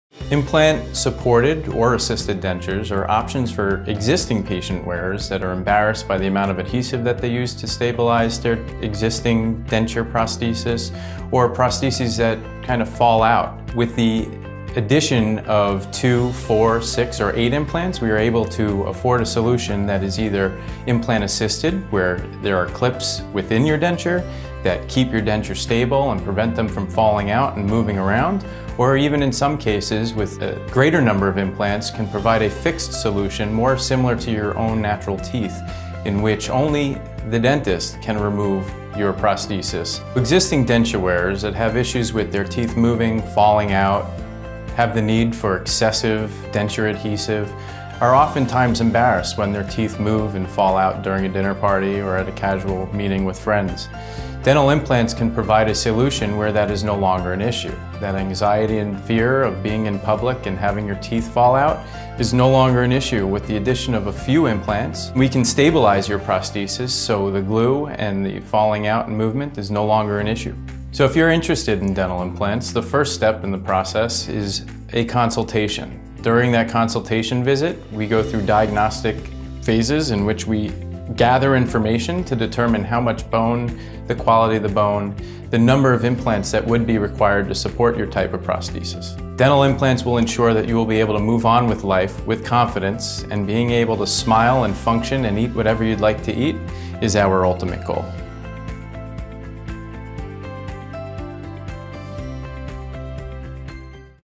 Dentures that stick without sticky adhesive!
Dr. Berg discusses implant supported dentures and how they compare to traditional dentures. Unlike traditional dentures with implant supported dentures the implants are placed directly into a patient's mouth which allows the dentures to be placed directly into their mouth. One of the main benefits is that the patient no longer needs to use adhesive to hold their dentures into place or worry about them falling out while eating or during conversation.  Here at Great Neck And Mid-Island Dental Associates we discuss the implant supported dentures process with each patient so they're aware and informed.